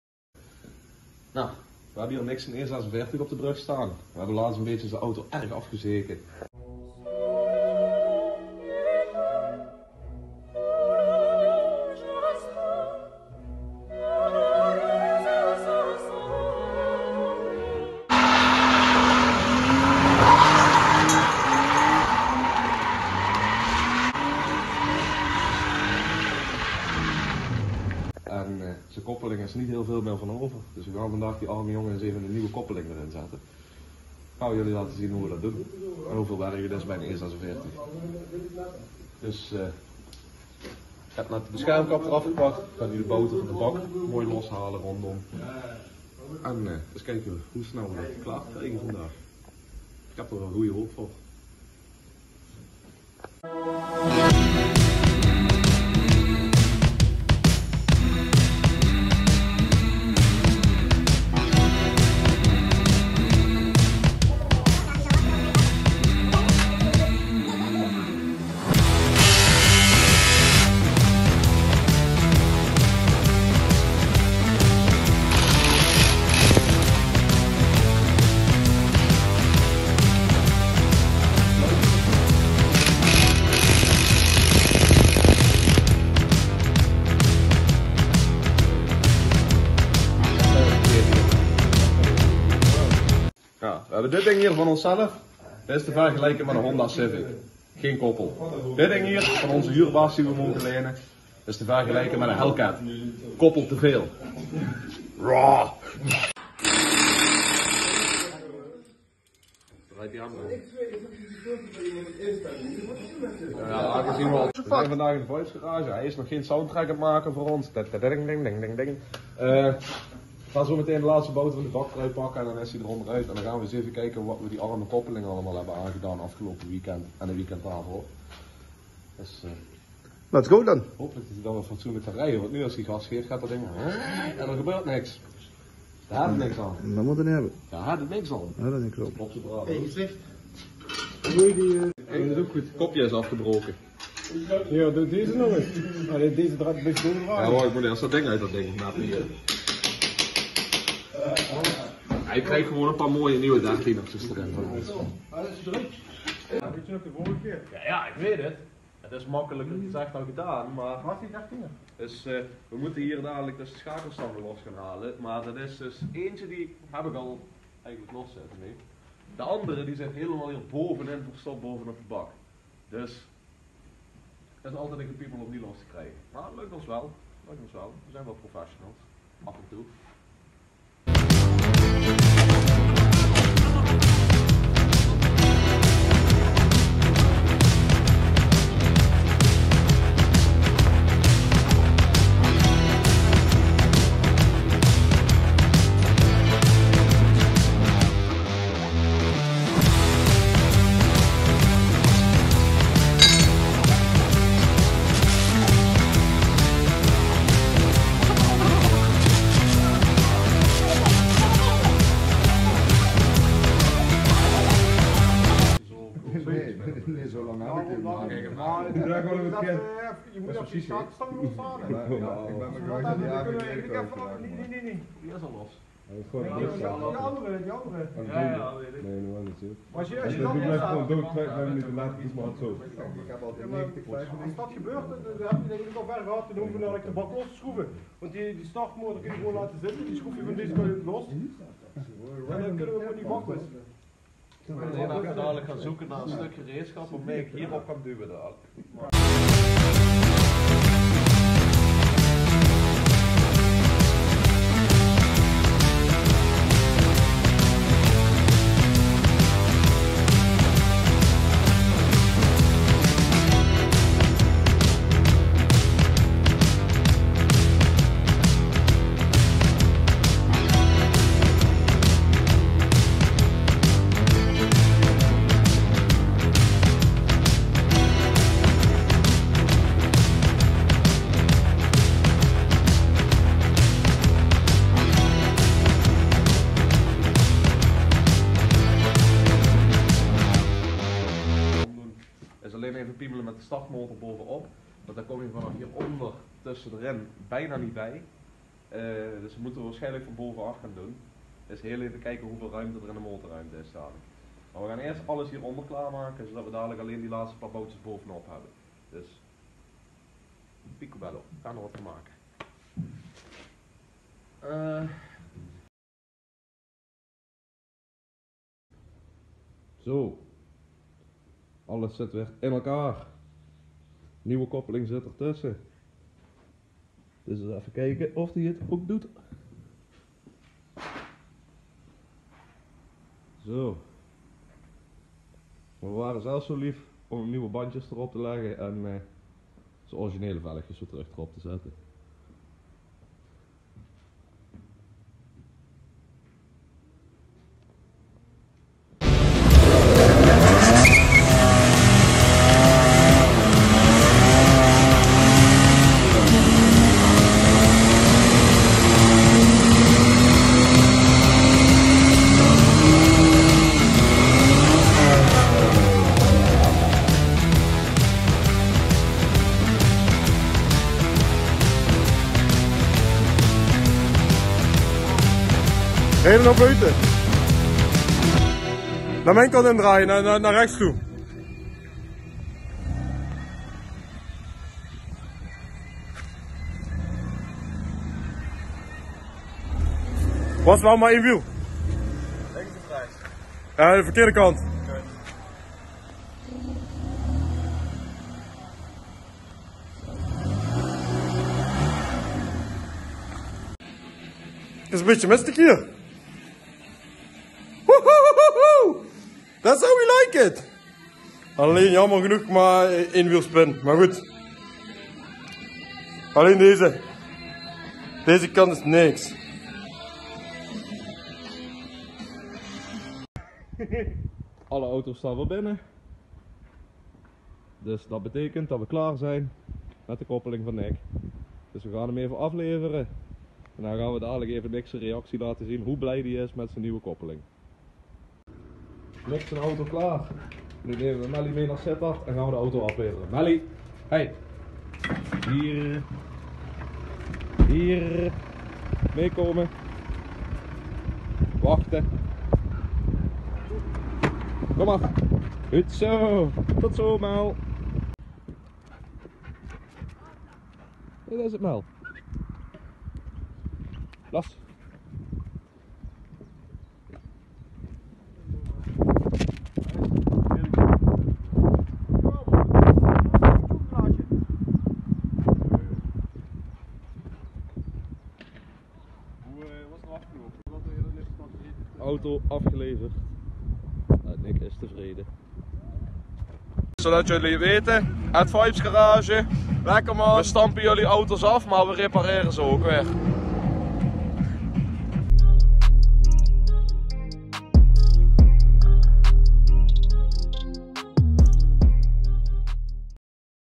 Replacing Nick's clutch on his E46 *After we wrecked it!* (Vlog #3)
Join us and watch how we put a new clutch in Nick his 325i E46 after we helped him wreck it the week before!